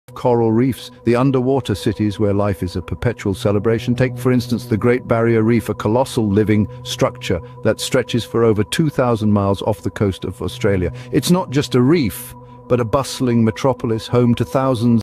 The Great Barrier Reef: Nature's Underwater Wonderland! 🐠
Dive into the mesmerizing world of The Great Barrier Reef, Earth's largest coral reef system! 🌊 Join us on an underwater adventure as we discover the vibrant marine life, breathtaking coral formations, and unparalleled beauty of this natural wonder. From colorful fish to majestic sea turtles, witness the wonders of one of the world's most iconic ecosystems. Don't miss this captivating journey through the heart of The Great Barrier Reef! GreatBarrierReef NaturalWonders CoralReef UnderwaterWorld 🌟